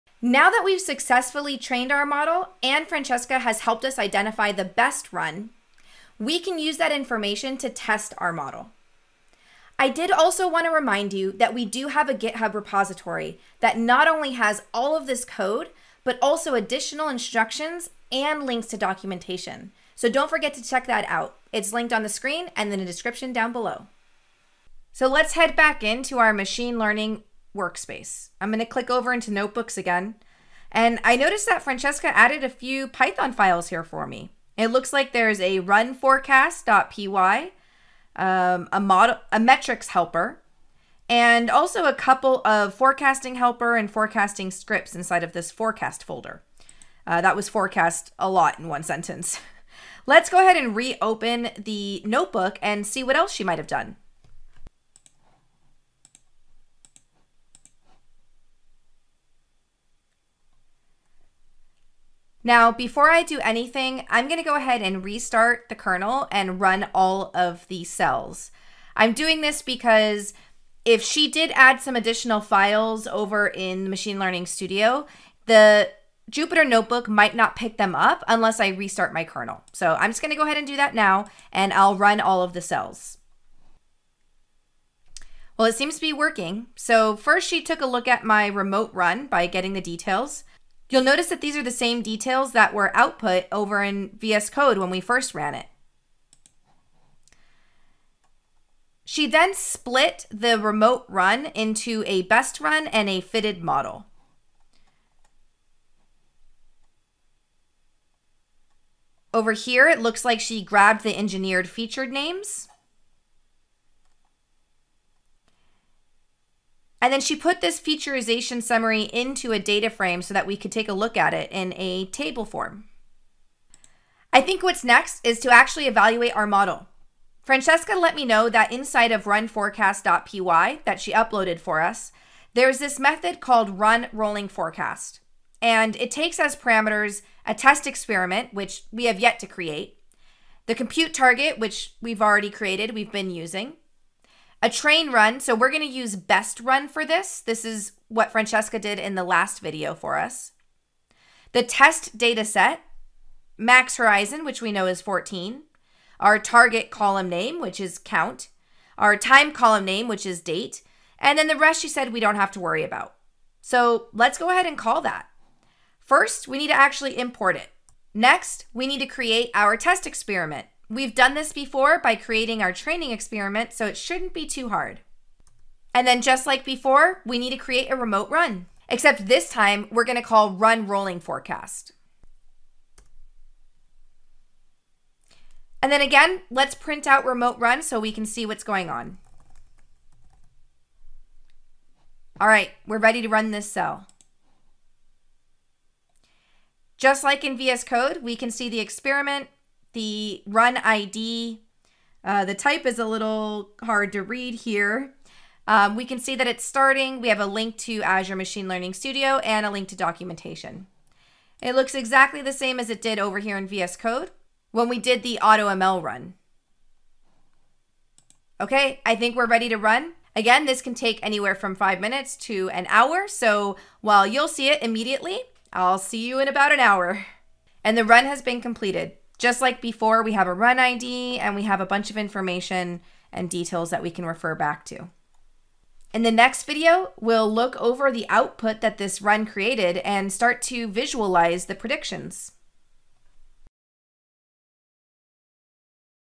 How do you evaluate and retrieve a time series forecast from Azure Machine Learning? (20 of 28)
In this video, you will learn how to use an external python function to run your data through a forecast evaluation. Using Python files uploaded to the cloud environment within the Azure Machine Learning Studio, you can call functions within those files from the Jupyter Notebooks within the same cloud environment.